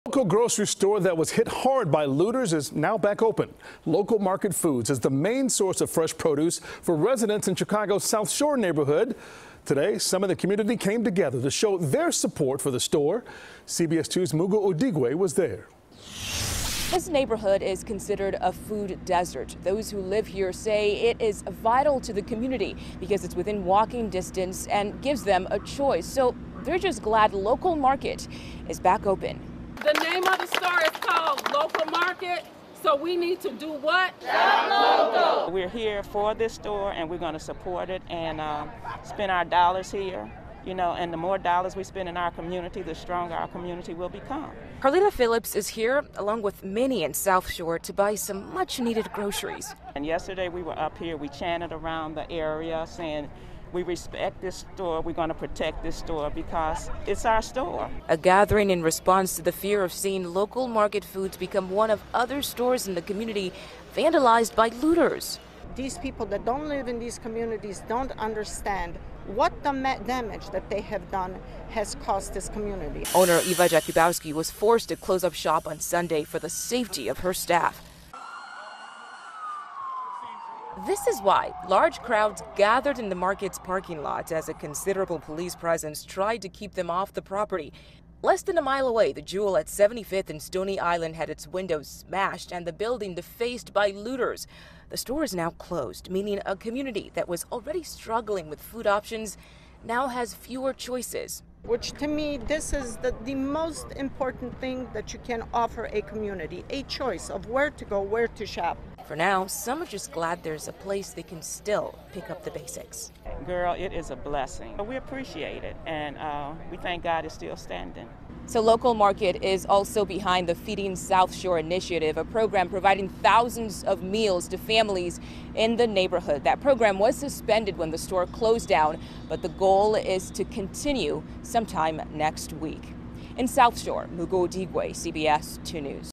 Community Comes Together To Support South Shore Grocery Store Hit By Looters
Local Market Foods is the main source of fresh food for people in Chicago's South Shore neighborhood, and the community came together to protect it.